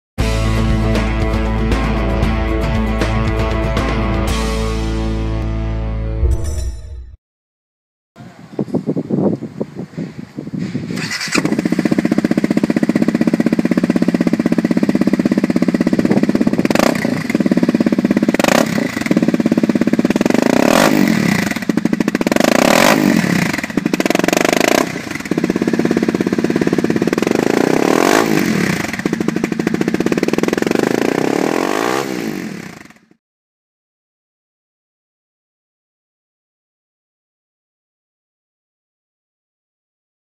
Honda CRF 250L Big Gun Exhaust EVO R System
From testing at international rally's around the world to local city streets, the Big Gun Exhaust EVO R slip on and full system were designed to add much needed performance and sound.

* Down spout included to clear OEM tail light assembly. Is not needed for many aftermarket tail light aasemblies. *

Slip On Part # 09-12582
Full System Part # 09-12583

Fits:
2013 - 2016 Honda CRF 250L